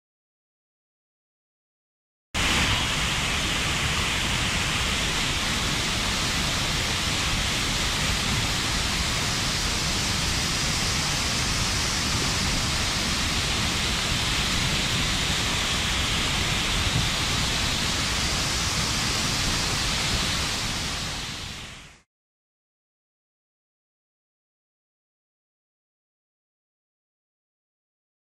La Fuentona - Soria (01/02/2019)
Así de espectacular se encontraba el pasado día 1 de febrero la Cascada de la Fuentona, tras las lluvias caídas los días previos.